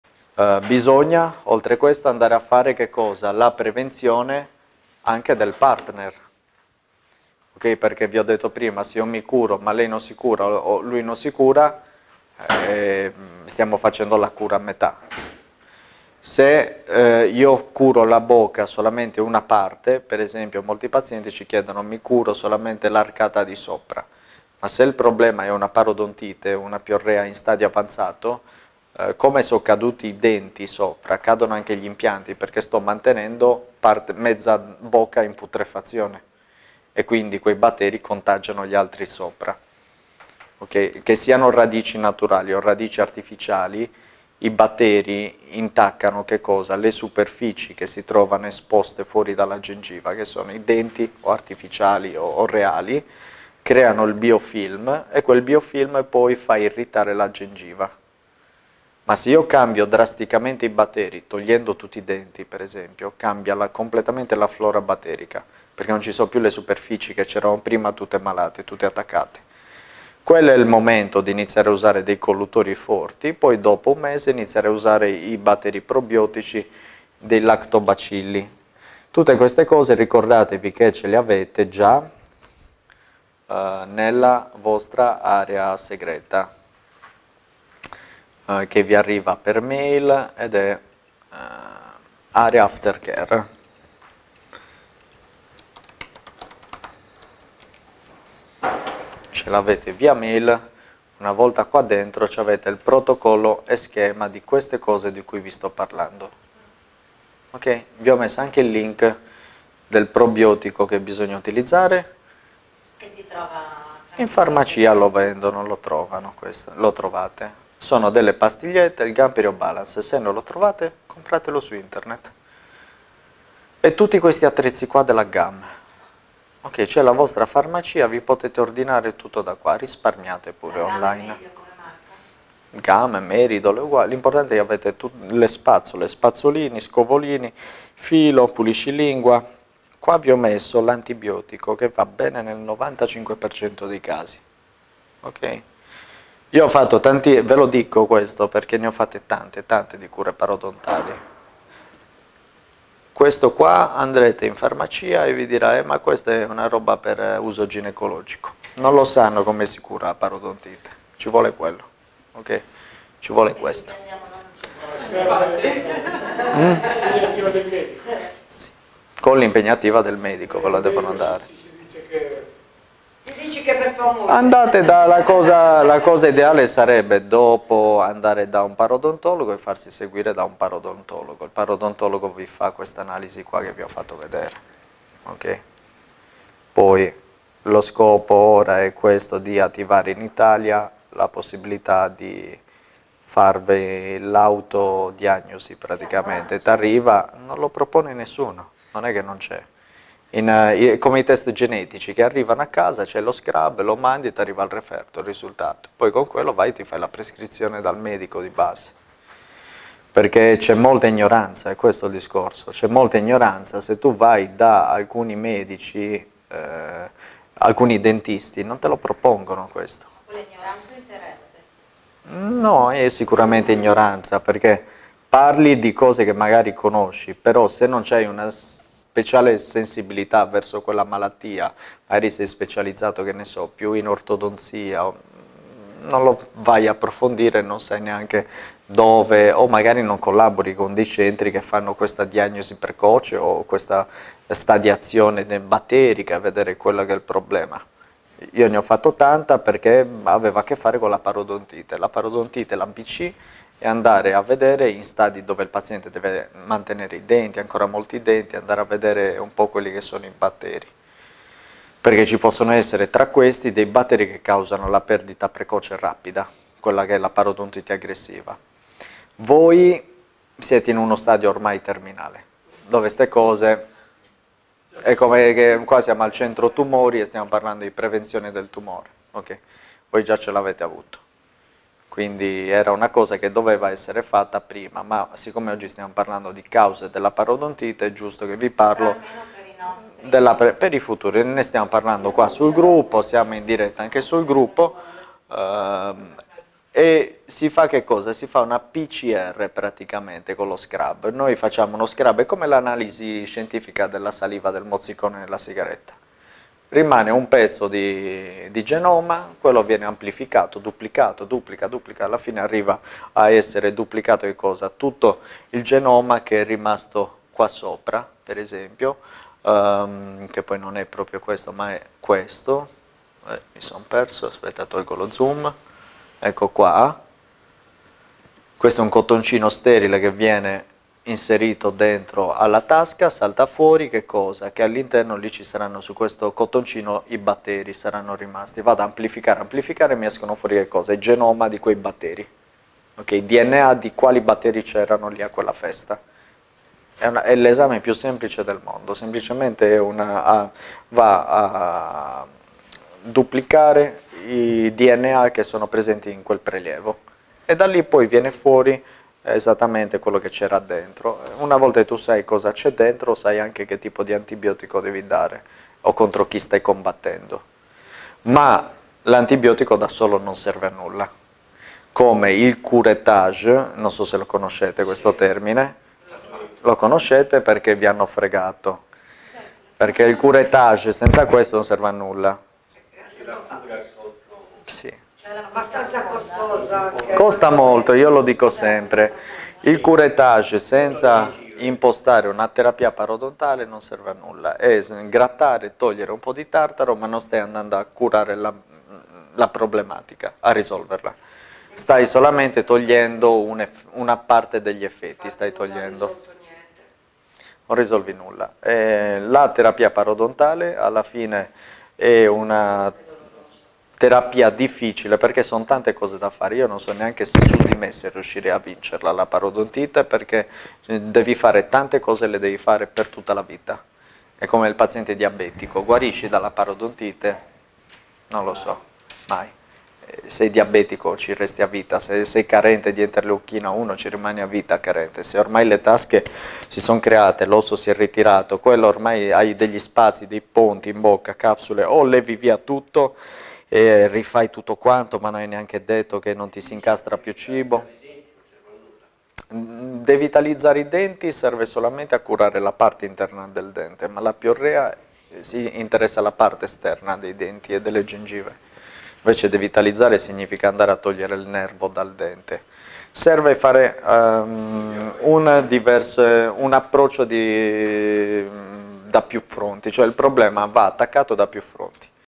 Qual è il miglior modo per curare la parodontite? | Denti fissi su impianti
Qual è il miglior modo per curare la parodontite (Piorrea)? Purtroppo la parodontite è una condizione spesso agevolata anche dalla genetica di una persona.

In buona sostanza la chiave per sbarazzarsi (o quantomeno mitigare tantissimo) il problema della parodontite è cambiare il microclima batterico della bocca.

Per i casi di cui ci occupiamo noi di SOS Turismo Dentale questo spesso comporta l'eliminazione delle poche radici malate rimaste in bocca - per poi procedere ad un intervento tipo all-on-4, all-on-6 o all-on-8.

OVVIAMENTE estrarre le radici restanti non è la giusta soluzione se queste radici sono ancora sane, in quel caso ovviamente bisognerà agire meno drasticamente data la minor gravità del problema - salvando così le radici. Non è però questo il caso tipico qui presso SOS Turismo Dentale.

Ti spiego più in dettaglio all'interno del video.